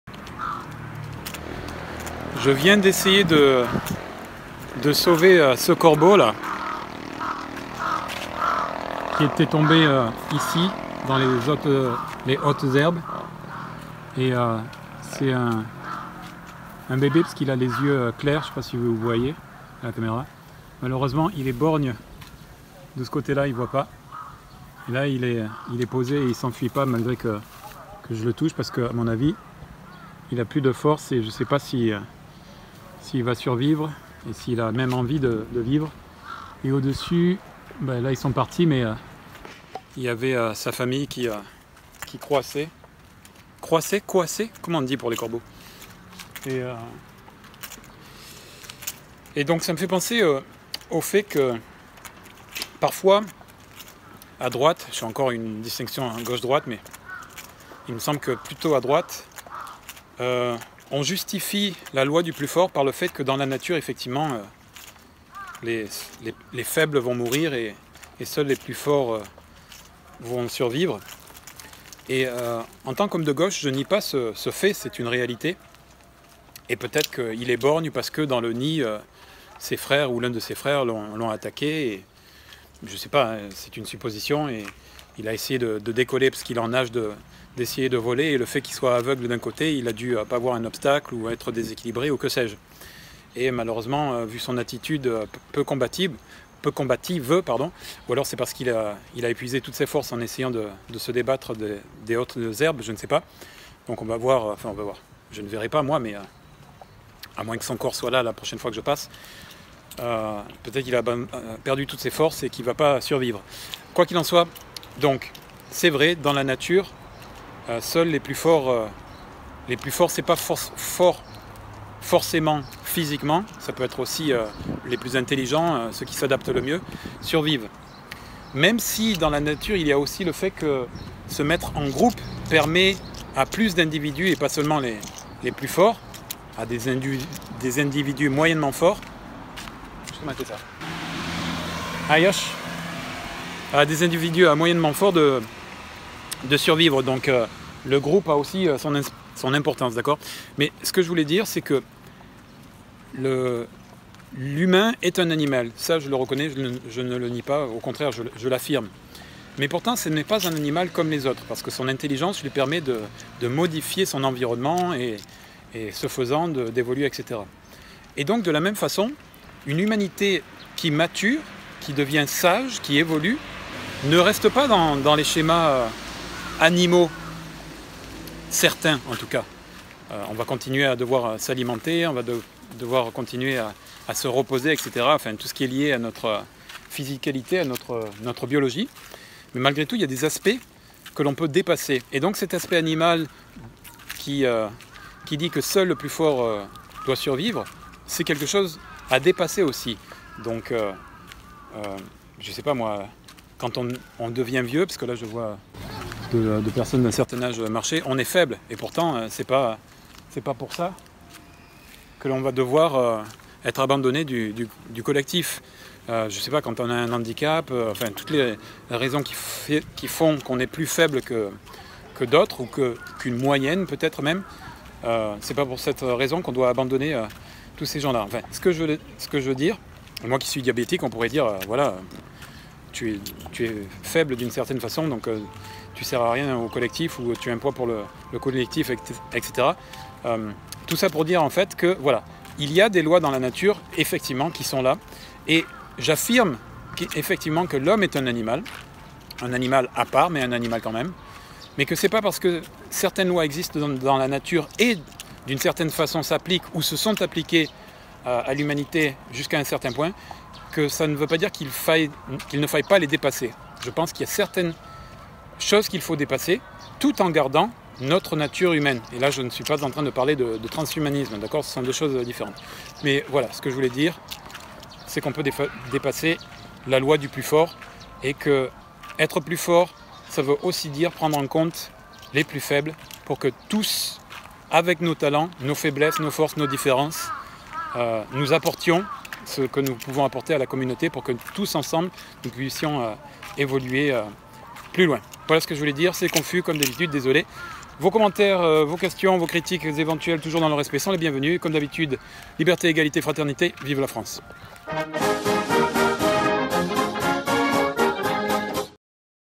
La Loi du Plus Fort
Souvent à droite l'inégalité est justifiée par le fait que la loi du plus fort est une loi naturelle.

Effectivement c'est bien une loi naturelle et l'homme est bien un animal. L'on pourrait donc penser que cette règle s'applique logiquement à notre espèce aussi.

Sauf que l'homme est un animal à part, doté d'une intelligence qui lui permet de dépasser sa simple condition animale, et d'évoluer différemment que les autres animaux.

Si certaines lois, ou conditions, doivent continuer à faire de nous ce que nous sommes, d'autres doivent être dépassées.

Que serait l'humanité si Einstein, considéré comme "faible" car juif par les nazis, n'avait pas pu apporter sa science ?

Idem avec Stephen Hawking, lourdement handicapé et pourtant génie de la science.

Pour ne citer que ces deux exemples bien connus.

Chacun, dans sa diversité, fusse-t-elle un handicap, une confession ou autres, a quelque chose à apporter à la communauté.

Ainsi donc, la force, la vraie, c'est de reconnaitre ce potentiel dans chacun d'entre nous. Nous tous, la communauté.